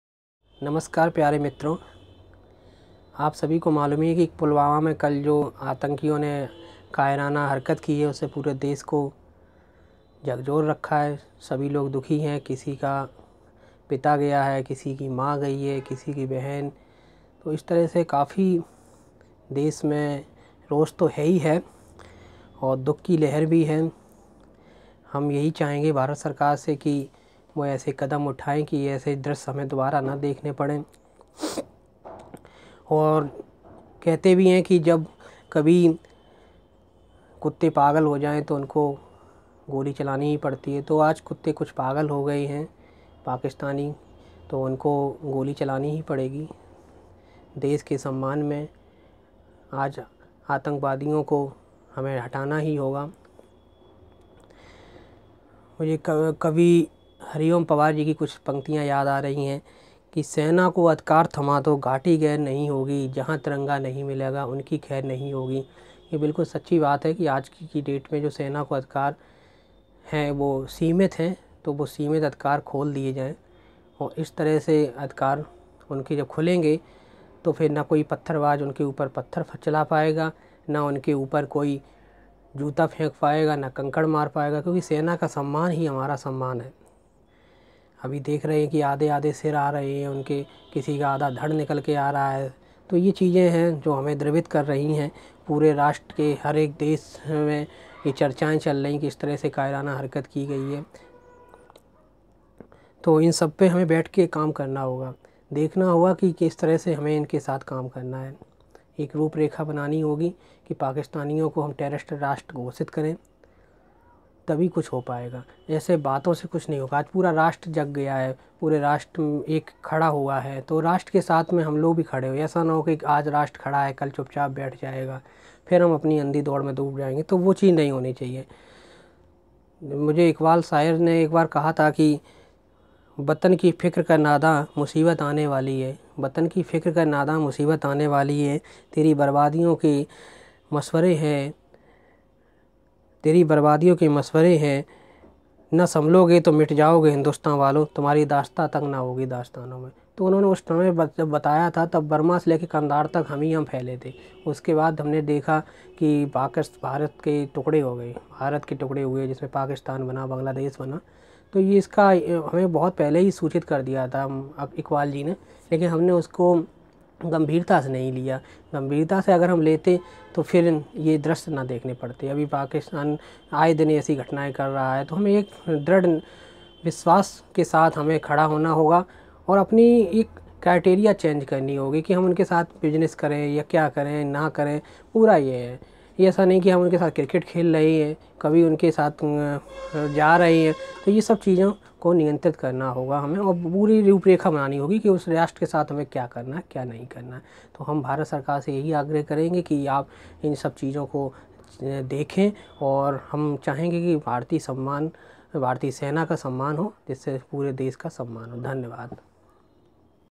Pulwama terror Attack
A tribute to all martyr of Pulwama Terror Attack. A request to Indian Govt. for stop such types of terror attack in future.